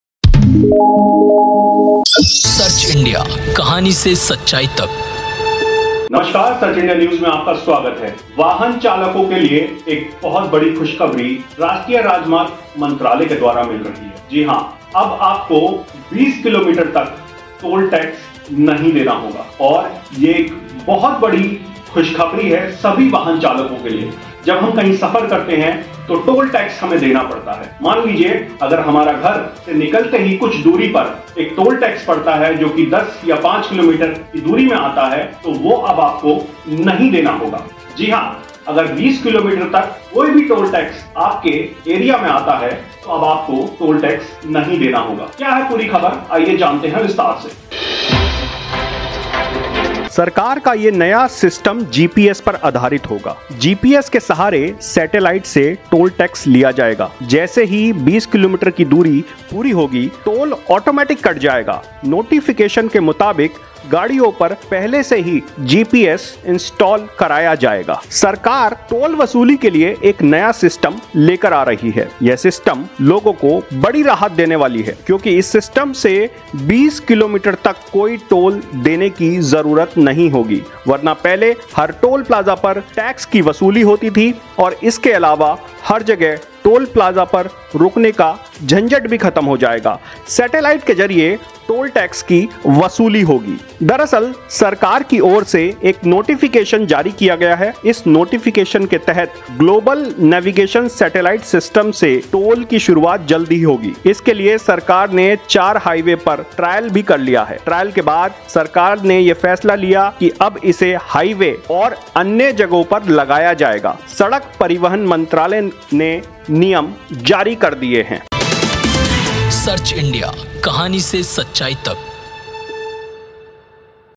वाहन चालकों को अब नहीं देना होगा टोल..!
गुड बाय FASTag? 20 किलोमीटर के दायरे में नहीं लगेगा टोल... आ गया नया टोल कलेक्शन सिस्टम.